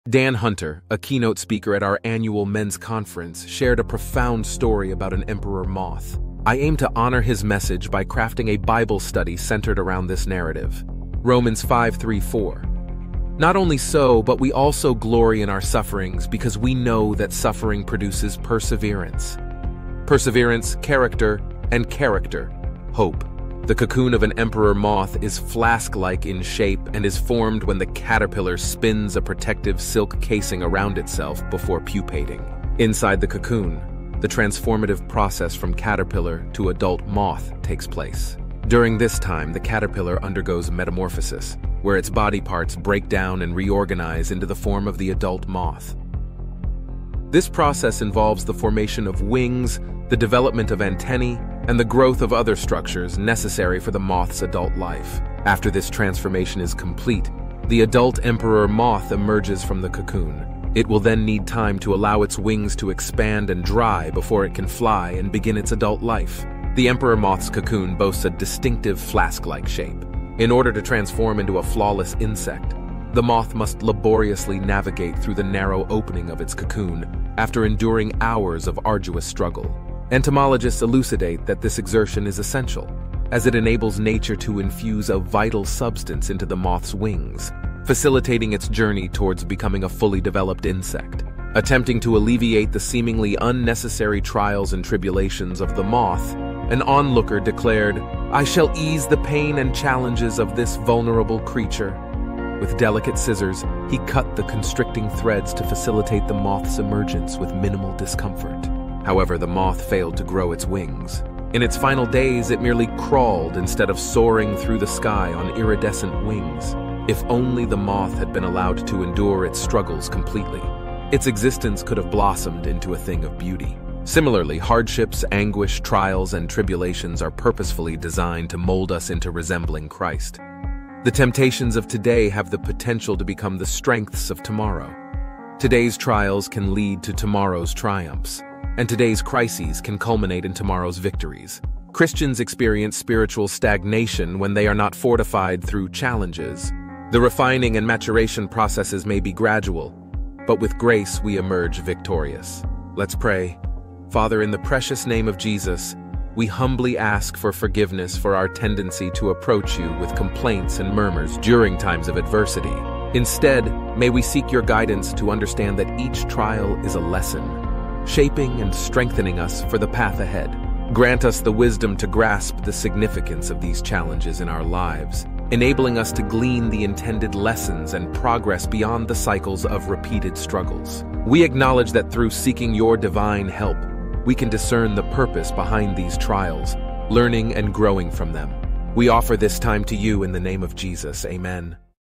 In His Image: Daily Reflection Lessons from the Emperor Moth
Dan Hunter, a keynote speaker at our annual men's conference, shared a profound story about an emperor moth. I aim to honor his message by crafting a Bible study centered around this narrative.

Romans 5:3-4: "Not only so, but we also glory in our sufferings, because we know that suffering produces perseverance; perseverance, character; and character, hope."

The cocoon of an emperor moth is flask-like in shape and is formed when the caterpillar spins a protective silk casing around itself before pupating. Inside the cocoon, the transformative process from caterpillar to adult moth takes place.

During this time, the caterpillar undergoes metamorphosis, where its body parts break down and reorganize into the form of the adult moth. This process involves the formation of wings, the development of antennae, and the growth of other structures necessary for the moth's adult life.

After this transformation is complete, the adult emperor moth emerges from the cocoon. It will then need time to allow its wings to expand and dry before it can fly and begin its adult life.

The emperor moth's cocoon boasts a distinctive flask-like shape.
In order to transform into a flawless insect, the moth must laboriously navigate through the narrow opening of its cocoon after enduring hours of arduous struggle. Entomologists elucidate that this exertion is essential, as it enables nature to infuse a vital substance into the moth's wings, facilitating its journey towards becoming a fully developed insect.

Attempting to alleviate the seemingly unnecessary trials and tribulations of the moth, an onlooker declared, "I shall ease the pain and challenges of this vulnerable creature!" With delicate scissors, he cut the constricting threads to facilitate the moth's emergence with minimal discomfort. However, the moth failed to grow its wings. In its final days, it merely crawled instead of soaring through the sky on iridescent wings. If only the moth had been allowed to endure its struggles completely, its existence could have blossomed into a thing of beauty. Similarly, hardships, anguish, trials, and tribulations are purposefully designed to mold us into resembling Christ.

The temptations of today have the potential to become the strengths of tomorrow; today's trials can lead to tomorrow's triumphs; and today's crises can culminate in tomorrow's victories. Christians experience spiritual stagnation when they are not fortified through challenges. The refining and maturation processes may be gradual, but with grace, we emerge victorious.

Lets pray...Father, in the precious name of Jesus, we humbly ask for forgiveness for our tendency to approach You with complaints and murmurs during times of adversity. Instead, may we seek Your guidance to understand that each trial is a lesson, shaping and strengthening us for the path ahead. Grant us the wisdom to grasp the significance of these challenges in our lives, enabling us to glean the intended lessons and progress beyond the cycles of repeated struggles. We acknowledge that through seeking Your divine help, we can discern the purpose behind these trials, learning and growing from them. We offer this time to You, in the name of Jesus. Amen.